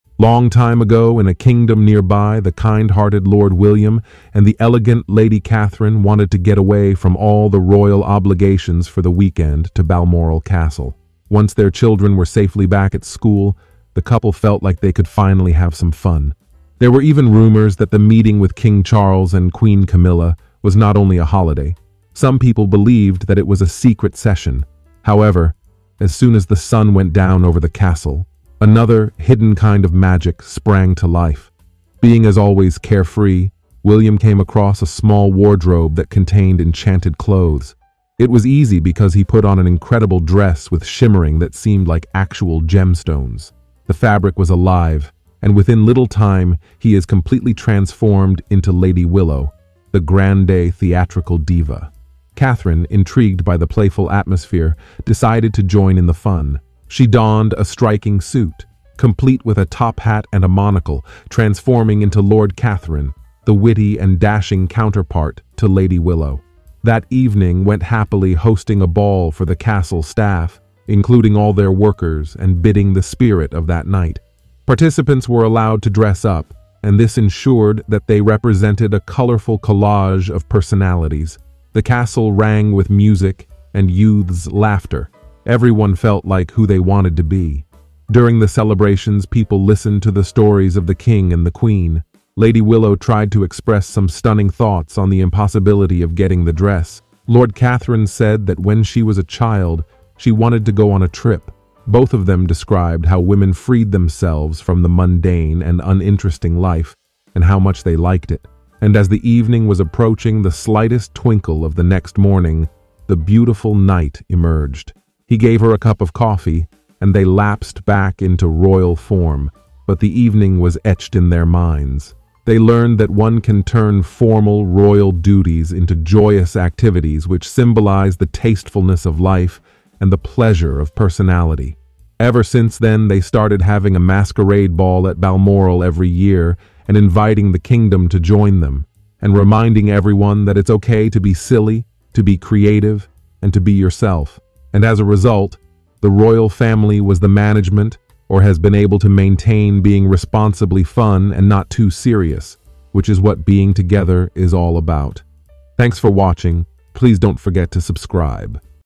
A Royal Transformation | Tg Tf | Gender Bender | Tg Captions
Releted Keywords

About this story

Lord William and Lady Catherine visit Balmoral Castle for their vacation getting away from all the royal drama. They come across a magic wardrobe that transforms William into a stunning beauty lady, willow and Catherine into a handsome lord, Catherine. An example of this is during a festive ball for their staff they encourage freedom of expression. As much as they have the fun in the evening they agree to make the masquerade an annual debate and also to remind the kingdom to be fun and be who you are.